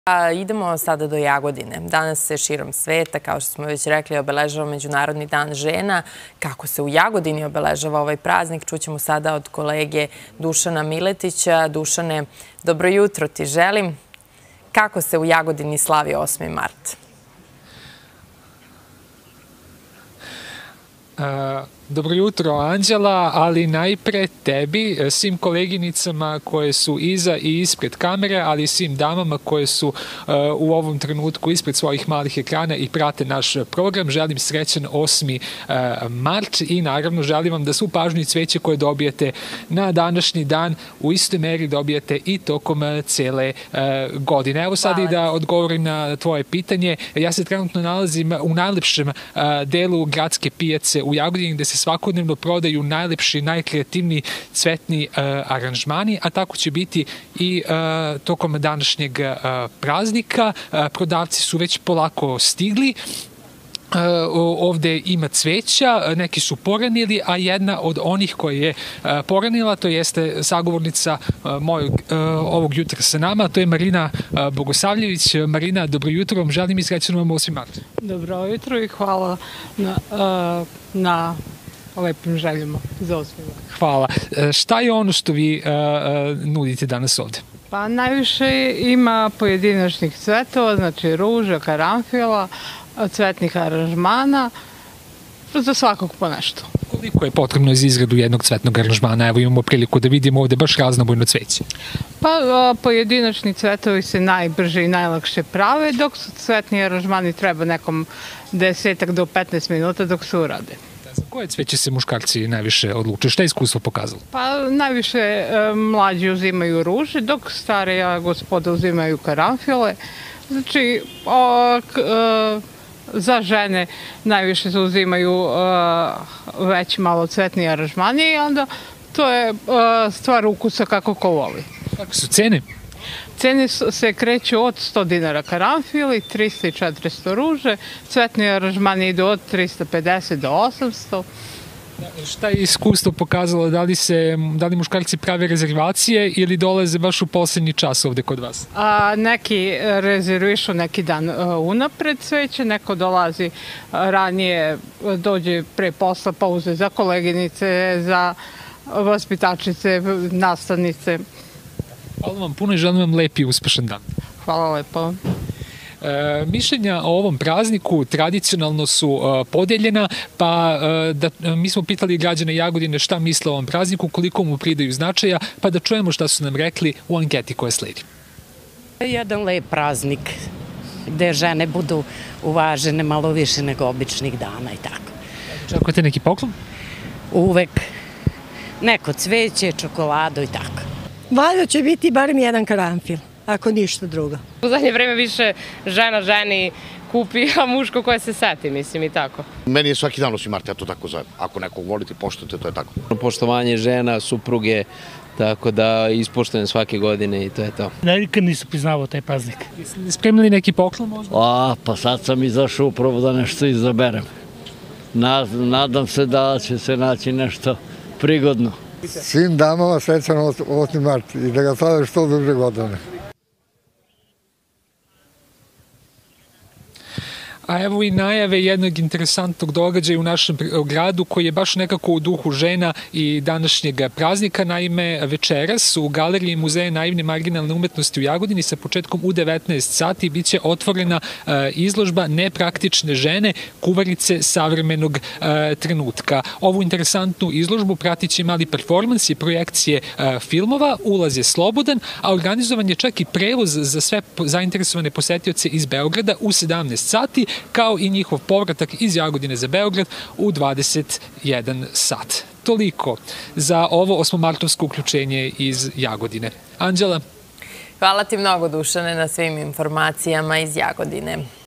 Jutarnje uključenje za TV PRVA - Kako će Jagodinci ulepšati svojim damama 8. mart?
Danas se obeležava Međunarodni dan žena. Evo kako su se pripremili prodavci cveća u Jagodini, a pogledajte i šta misle građani Jagodine o ovom prazniku.

Reporter: Dušan Miletić
Snimatelj: Saša Jović
proizvodnja: Kopernikus RTV Jagodina